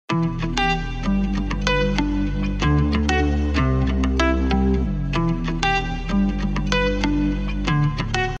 SWAT Media Group Inc. | Media Solutions
We are SWAT Media. A team specializing in taking your advertisements and ensuring they reach your intended audience.
Don't have a marketing team? You can borrow ours. Let's get started.